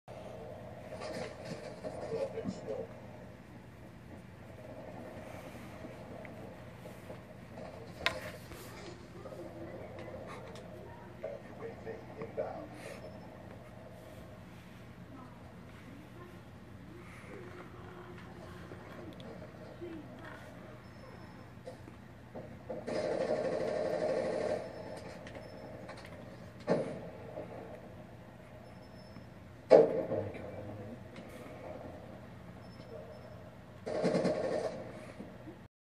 Cat doesn't like the company
Well, he doesn't look like he's enjoying himself. His name is Shadow, and he's 11 years old.